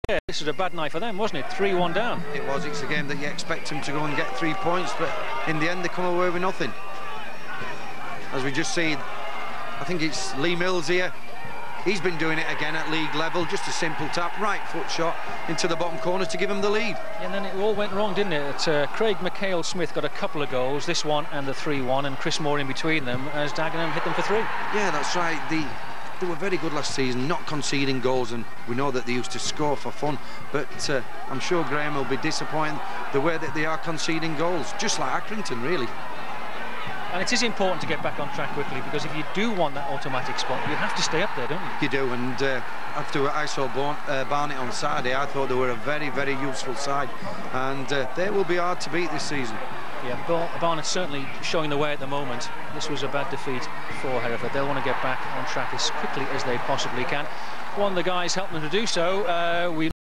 Dagenham & Redbridge 3-1 Hereford United | 5th October 2004
Line Ups
Dagenham
1 Roberts
2 Cole
3 Vickers
4 Foster
5 Janney
6 Southam
7 Mackail-Smith
8 Uddin
9 Flynn
10 Bruce
11 Moore
Sub Barton
Hereford
1 Gould
2 Travis
3 Tretton
4 James
5 Smith
6 Robinson
7 Williams
8 Hyde
9 Stanley
10 Mills
11 Brown
Subs Stansfield, Purdie
Attendance 1,417